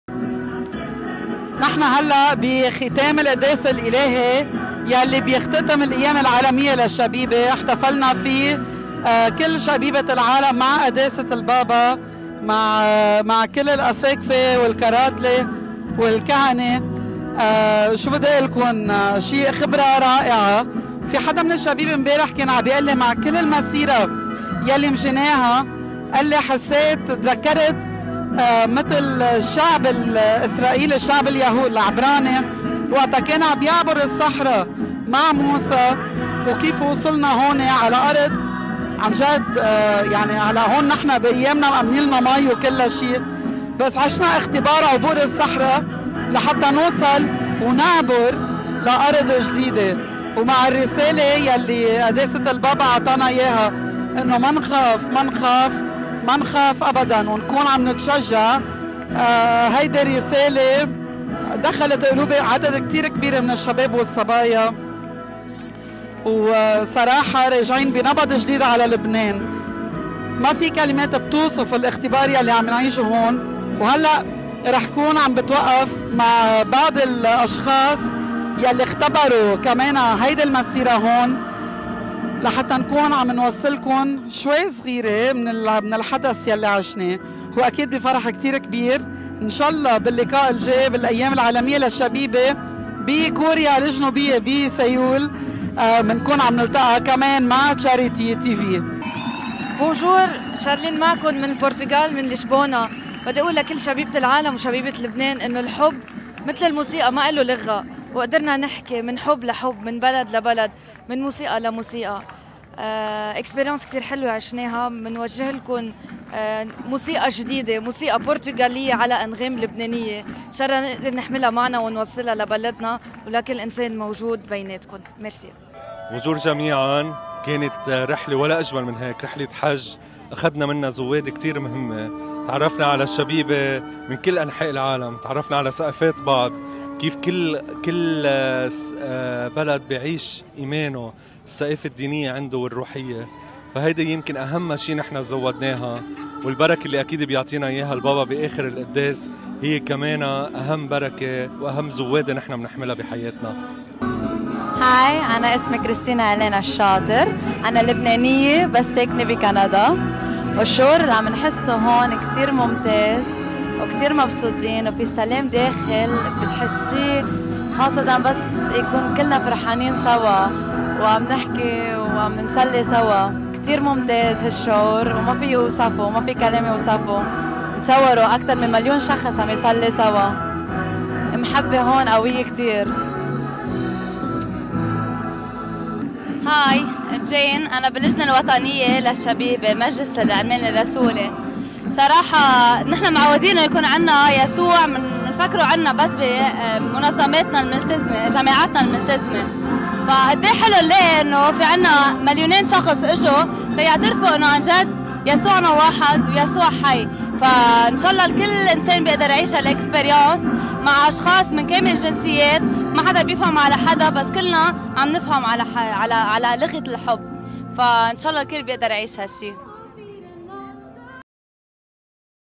JMJ 2023 - كيف عاش شباب لبنان الأيام العالمية للشبيبة في لشبونة 2023
إختبارات شبيبة لبنان في تقرير نهائي عن الأيام العالميّة للشبيبة في لشبونة 2023.
كيف عاشت شبيبتنا هذه الأيام؟